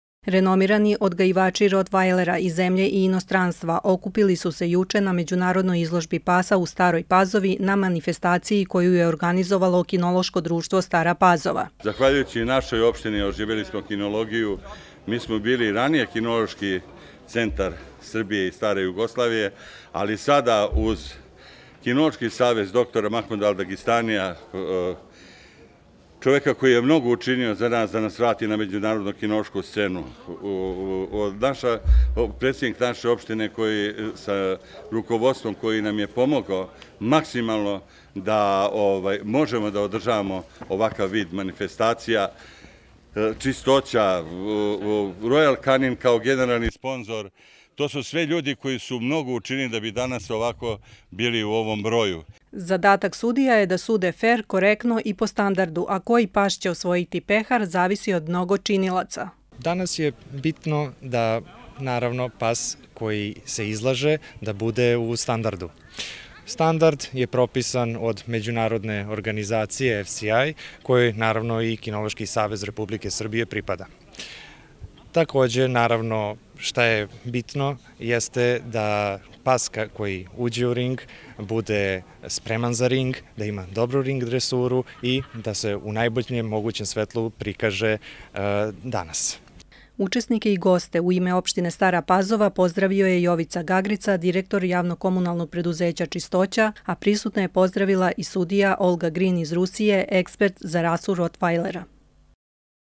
CACIB
Međunarodna kinološka manifestacija CACIB, specijalizovana za rasu rotvajlera, održana je juče na rasadniku „Brest“ u Staroj Pazovi.